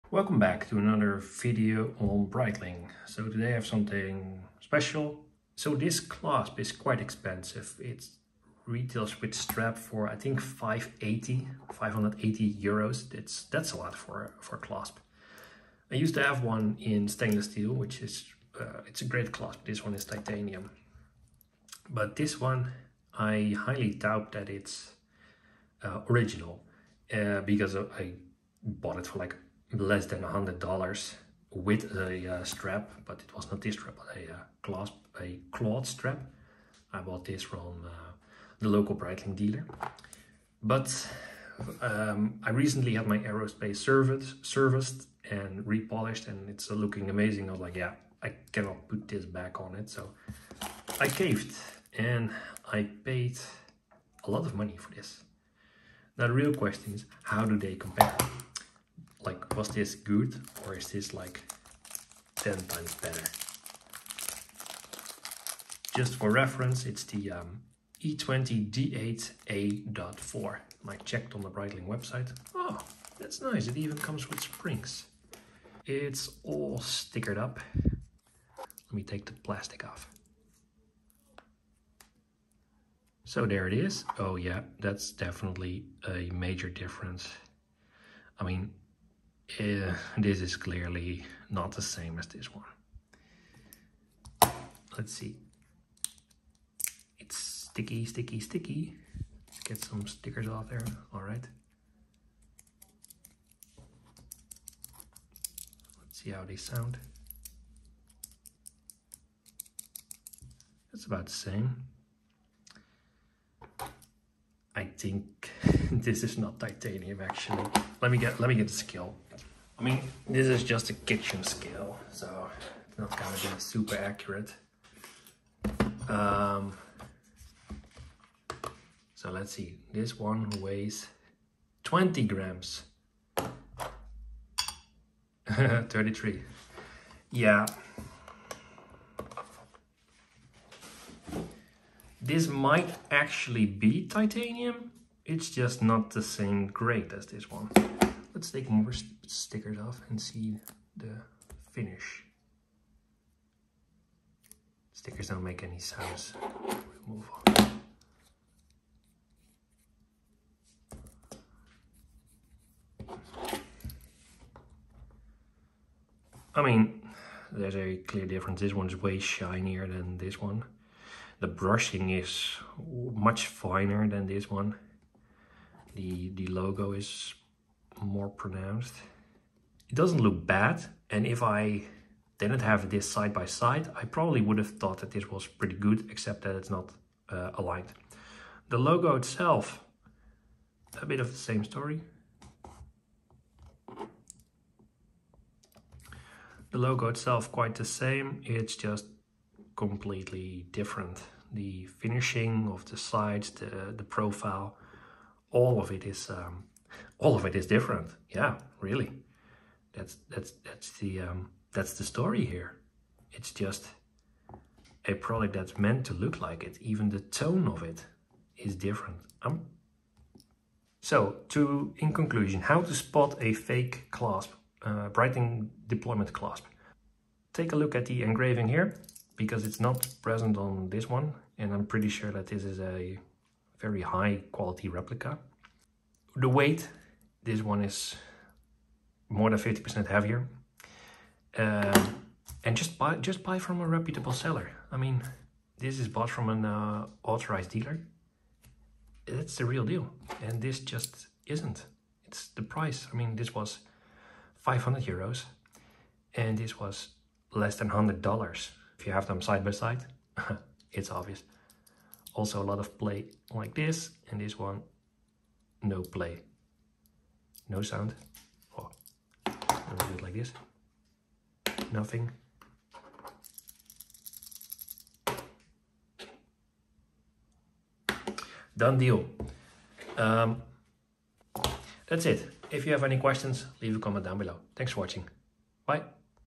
How to spot a fake Breitling clasp
Don't get fooled by scammer, watch this video and you will know the difference between a real and a fake Breitling clasp.